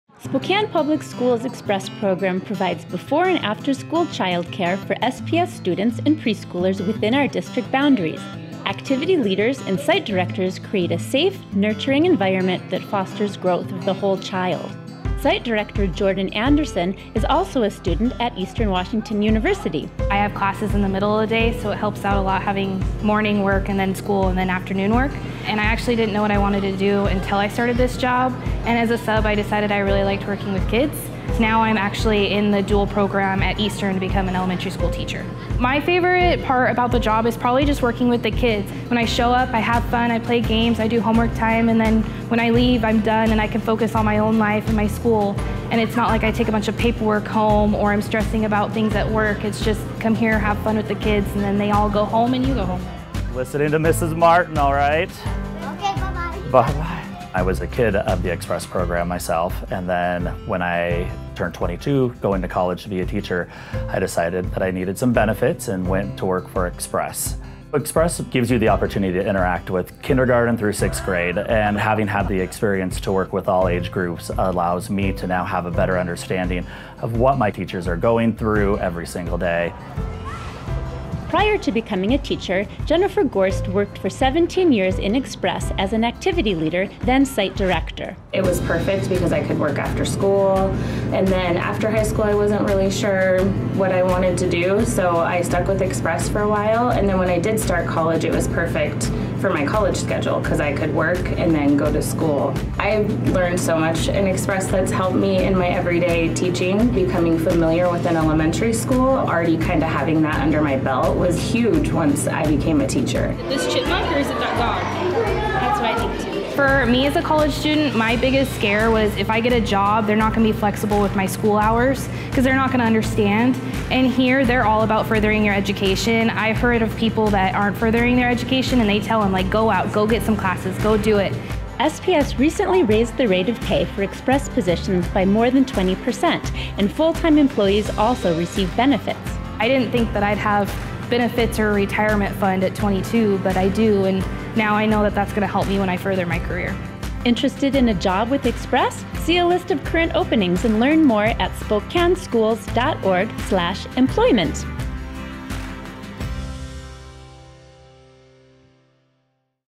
Why should you work for our Express Childcare program?
Spokane Public Schools' Express program provides before and after school childcare for SPS students and preschoolers within our district boundaries. Activity leaders and site directors create a safe, nurturing environment that fosters growth of the whole child.